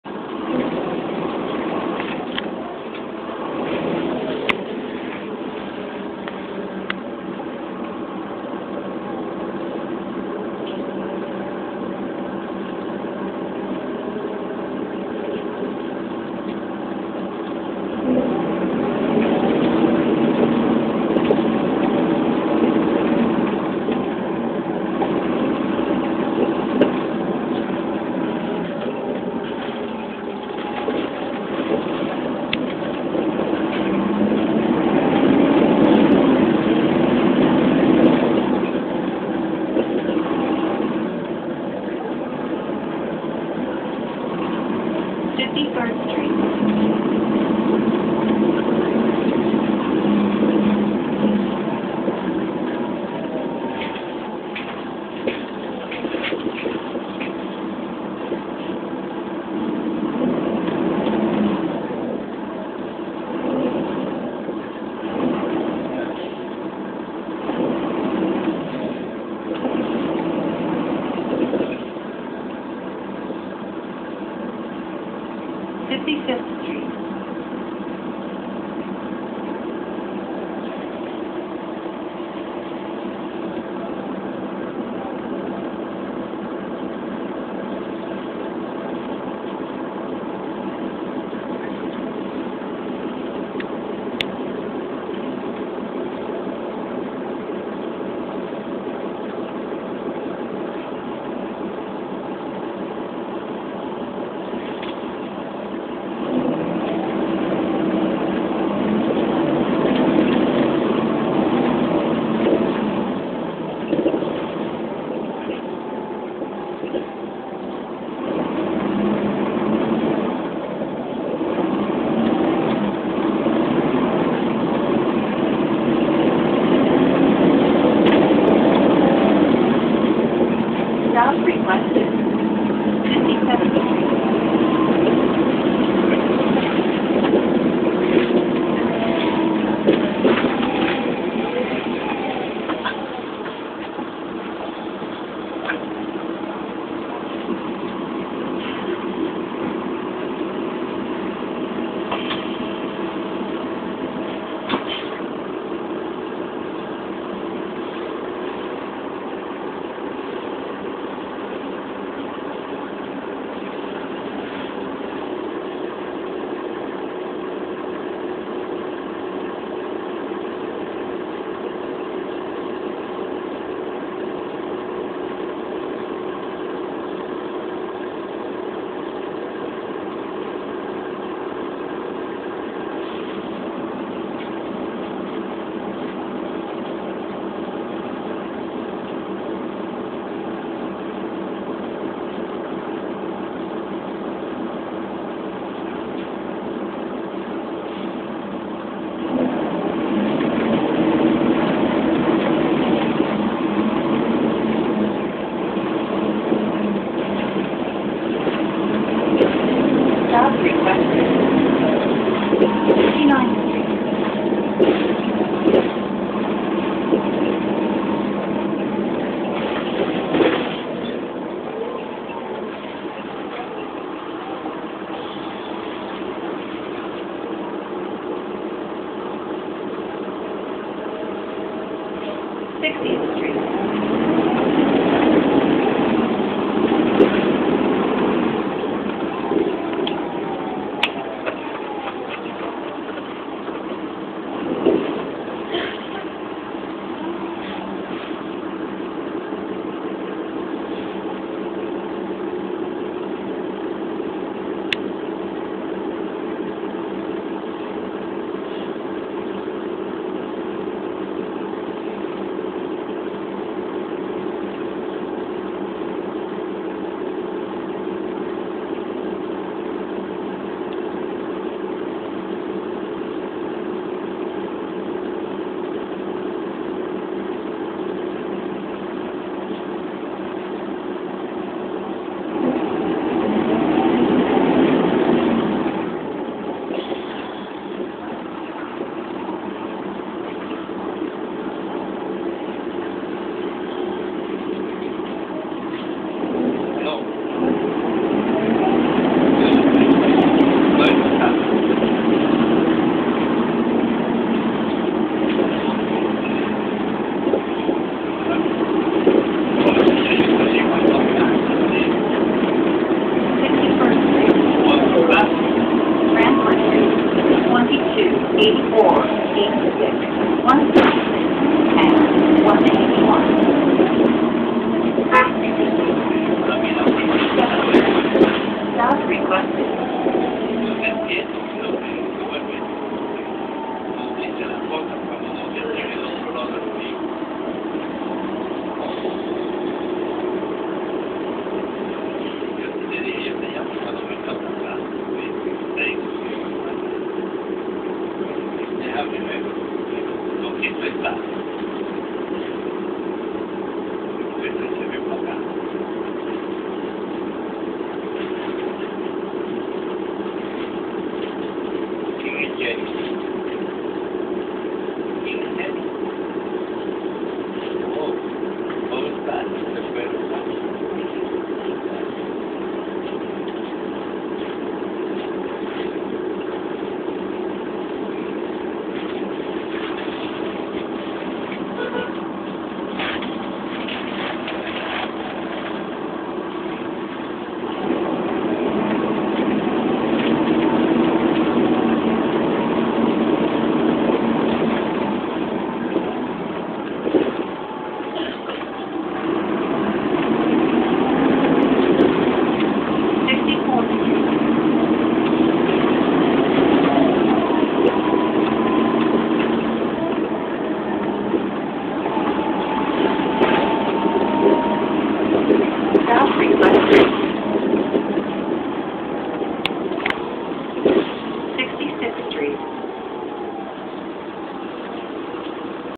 NJT NABI 40-SFW #5239 on the 159 to Fort Lee (Inside)
A 4th video ride on-board NJT 2009 (North American Bus Industries) NABI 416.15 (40-SFW) Suburban-Style #5239 on the 159 to Fort Lee and Linwood Park on an early Monday Afternoon.  I sat in the back once again and this video was recorded from my Samsung Gravity T Cell Phone.